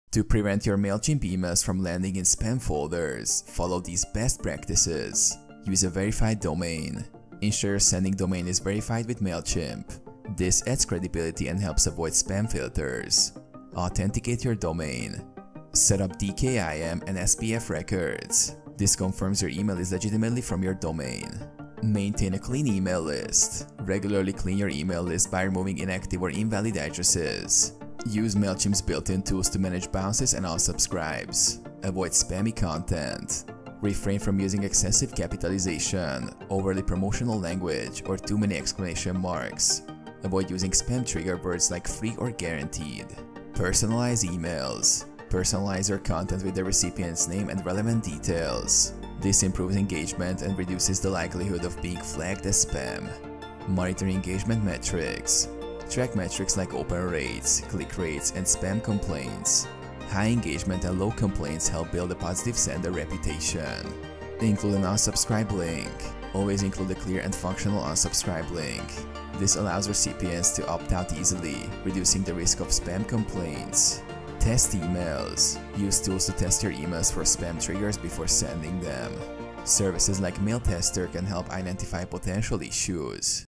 How To Avoid Mailchimp Going To Spam? (2024)
In this video I will explain How You Can Avoid Mailchimp Going To Spam In 2024.